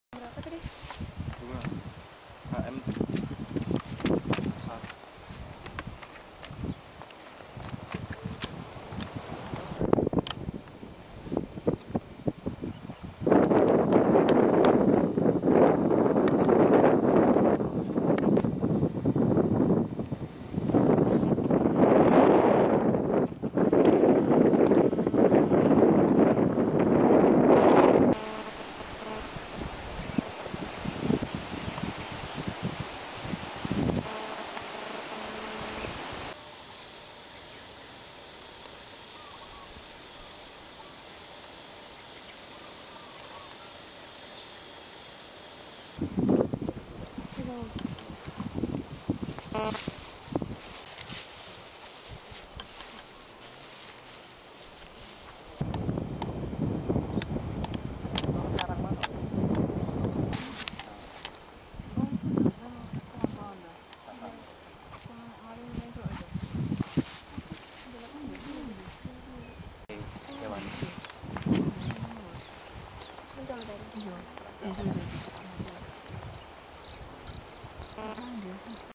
Birding Competition 2012 At TN. Baluran (2)
Berwisata sambil mengamati burung di alam bebas, mungkin menjadi kesenangan tersendiri bagi para pencinta alam. Taman nasional baluran, situbondo jawa timur kembali menggelar lomba mengamati burung (birdibng competition), semua jenis burung yang ada dalam kawasan hutan seluas 25 hektar itu.

Selain itu. Acara ini bertujuan melindungi burung satwa yang ada, yang mulai terancam kepunahan.